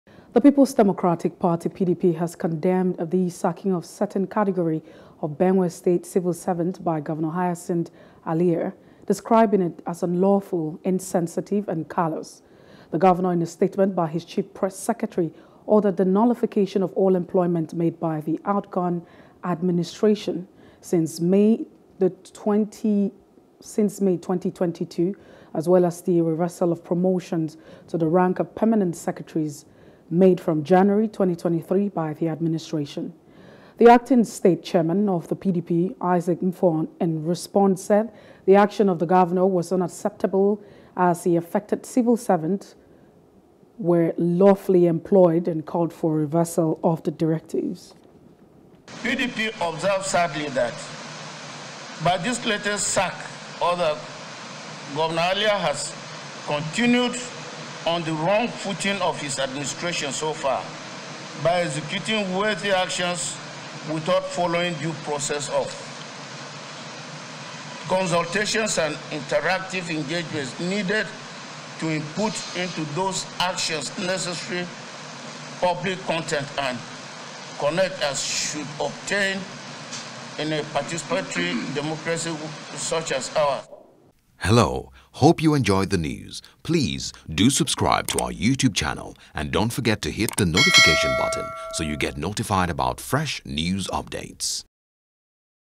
Benue: PDP Slams Gov Alia Over Sack Of Civil Servants
The People’s Democratic Party, PDP, has condemned the sacking of certain category of Benue civil servants by Governor Hyacinth Alia describing it as unlawful, insensitive and callous.